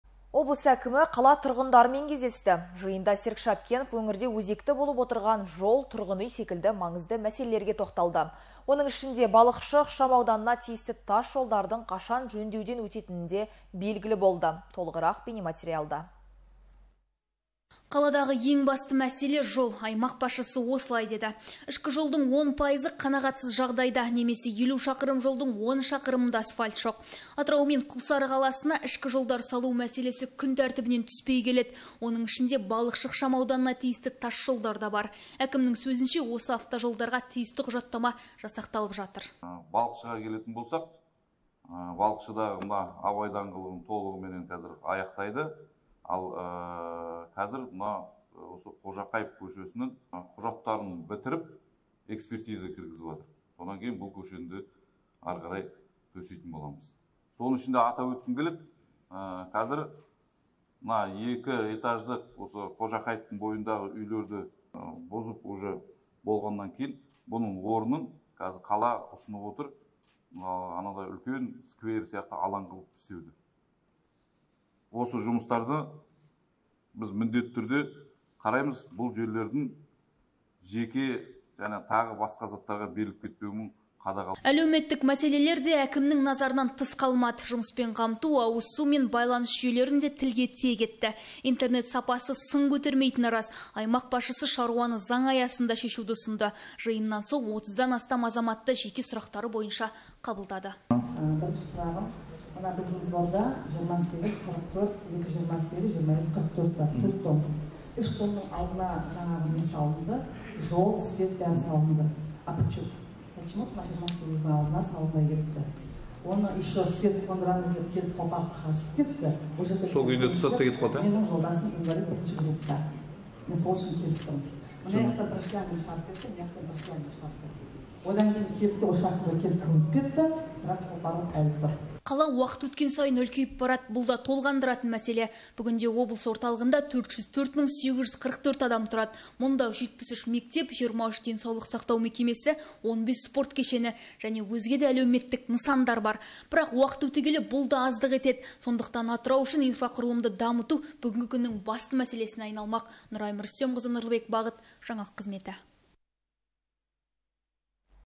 ОБЛЫС ӘКІМІ ТҰРҒЫНДАРМЕН КЕЗДЕСТІ
Облыс әкімі қала тұрғындарымен кездесті. Жиында Серік Шәпкенов, өңірде өзекті болып отырған жол, тұрғын үй секілді маңызды мәселелерге тоқталды. Оның ішінде Балықшы ықшам ауданына тиісті тас жолдардың қашан жөндеуден өтетінітде білгілі болды. Толығырақ бейнематериалда.